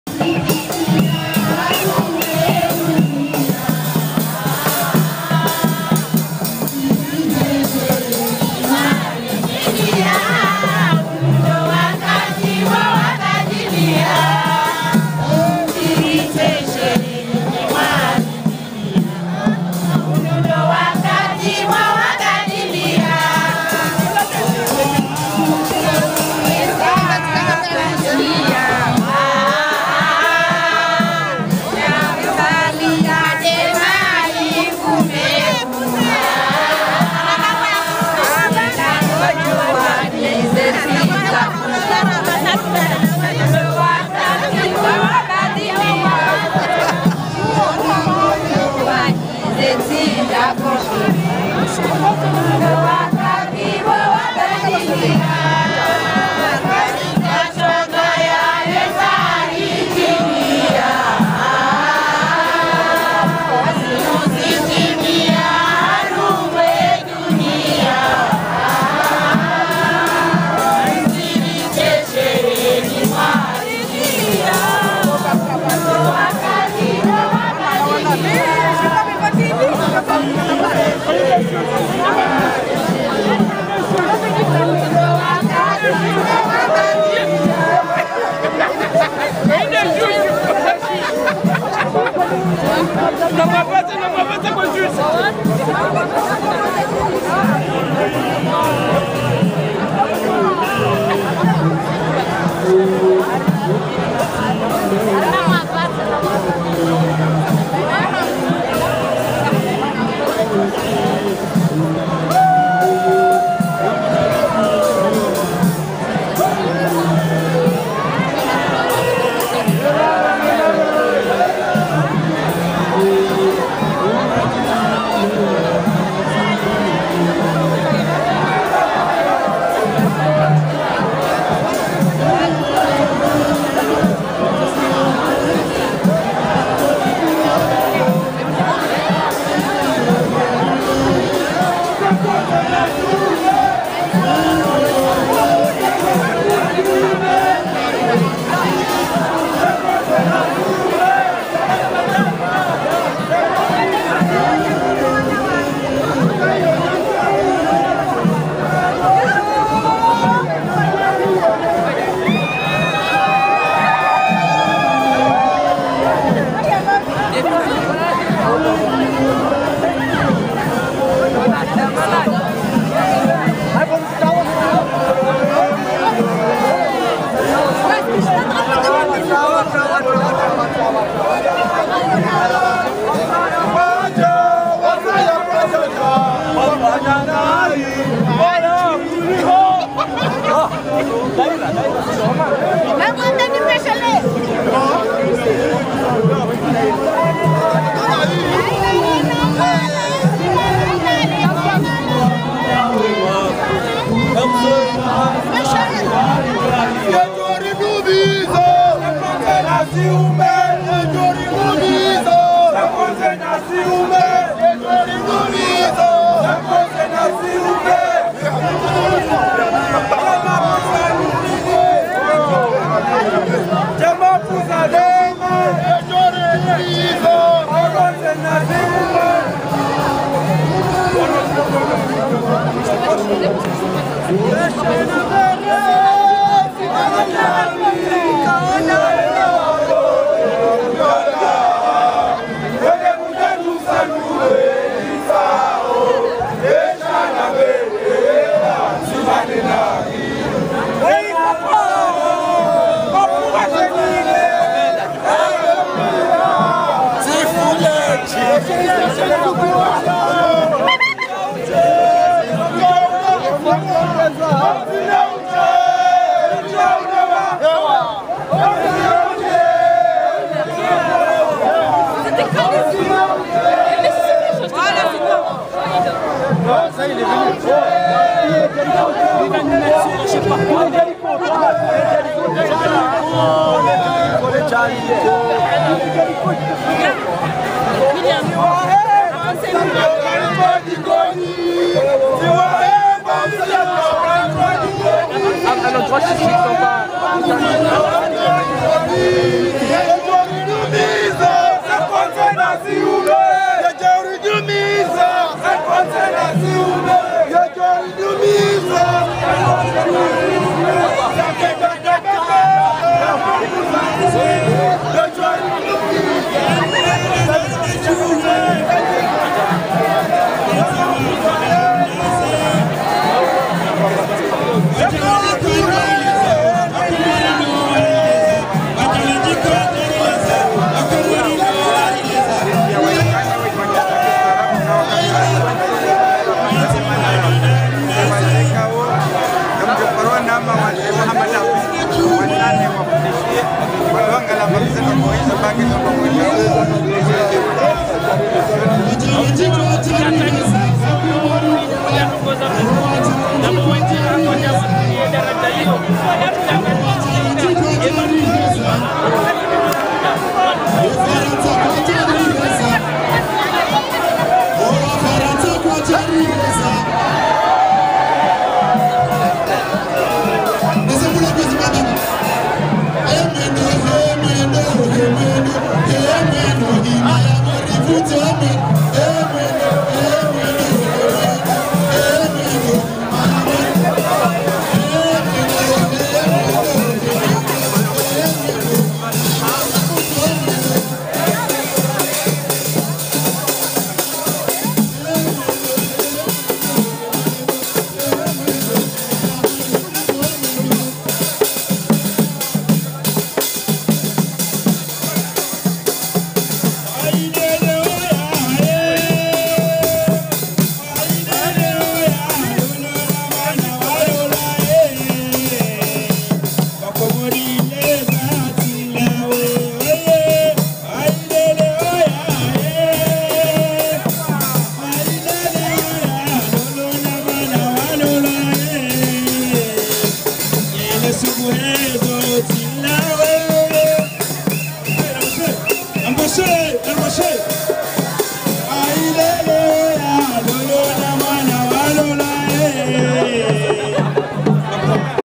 Défilé marseillais pour la Fête nationale des Comores - lemeilleurdemarseille.fr
Au bas de la Canebière un groupe de Marseillais d'origine comorienne se rend en dansant et chantant à l'Hôtel de ville à l'occasion de la Fête nationale des Comores